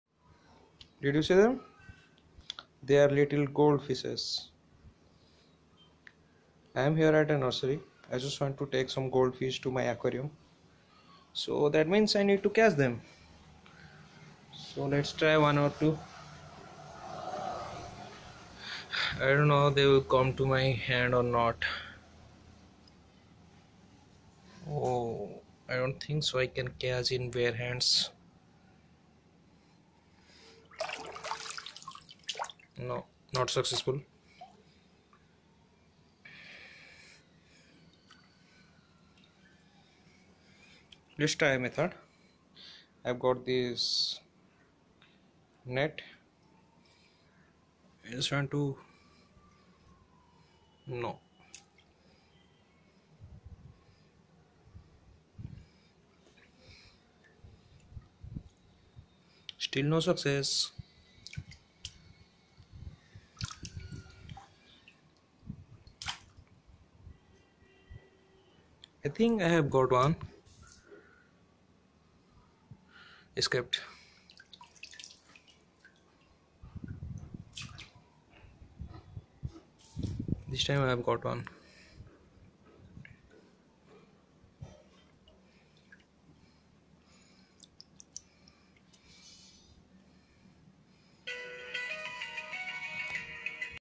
How to catch fish from Aquarium easily without Net
Catching golden fish from my little well